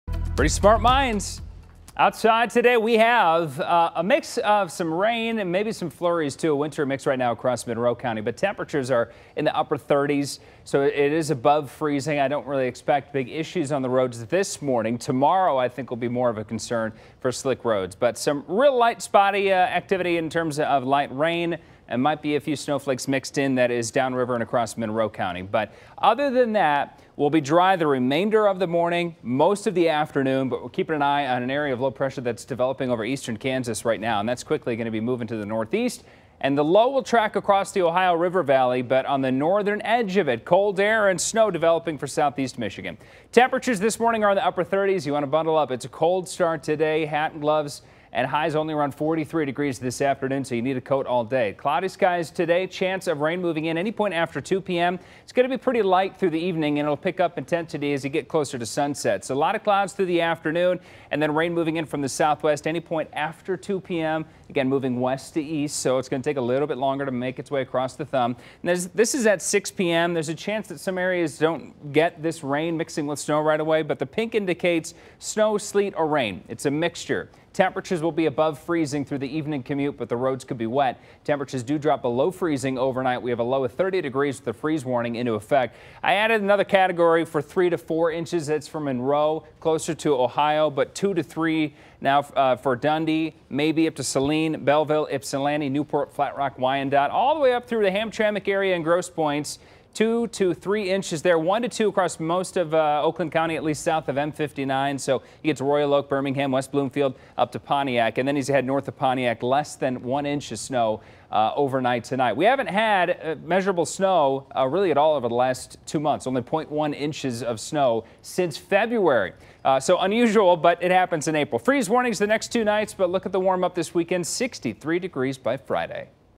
Metro Detroit Forecast: Freeze warnings and snow tonight
Cold and cloudy today with rain moving in later in the afternoon. Rain will change to snow tonight. Freeze warnings will be in effect the next two nights.